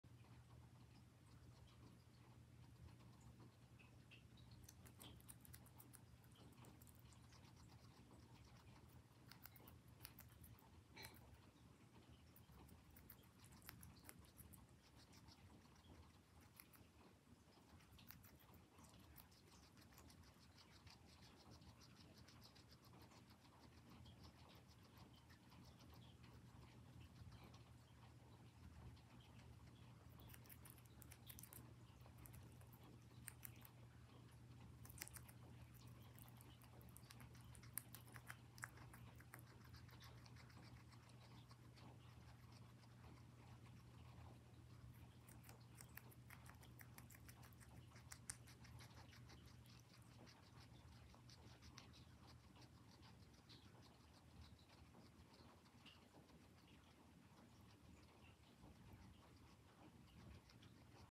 Does Baby Guinea Pig Butter like tomatoes? Let’s find out….
Butter, you are so cute!

Coco, Nugget, Butter and Poe love you!,

This video was edited with InShot